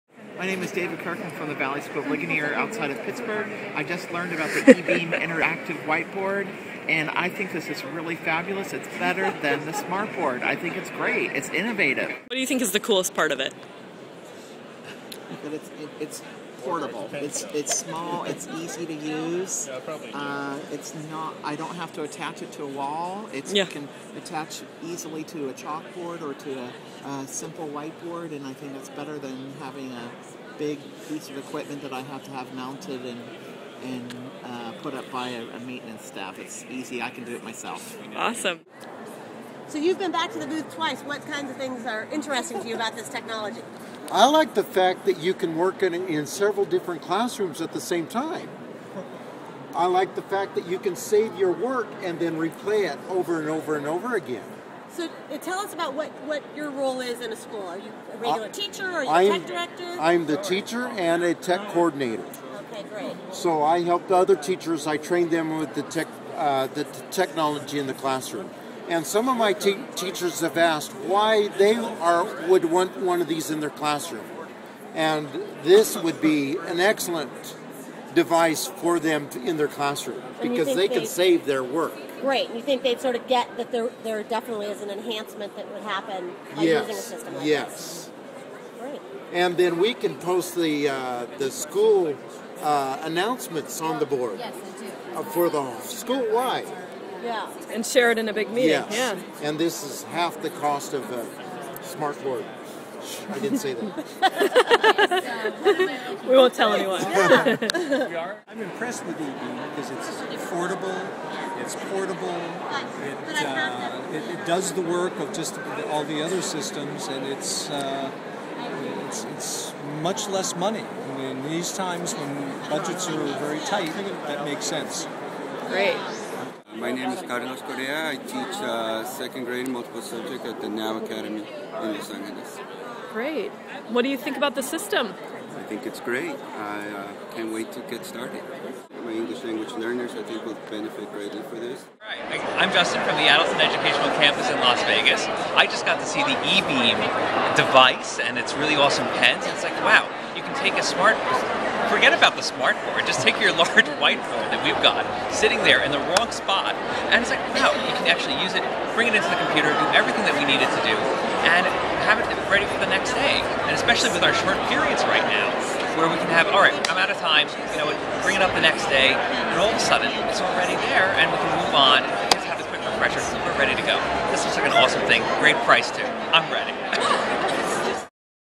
eBeam - Intial Reactions at NAIS & CUE 2010
Initial reactions from tech directors and teachers upon seeing eBeam interactive whiteboard systems in action at the National Association of Independent Schools conference in San Francisco, CA and the Computer Using Educators conference in Palm Springs, CA.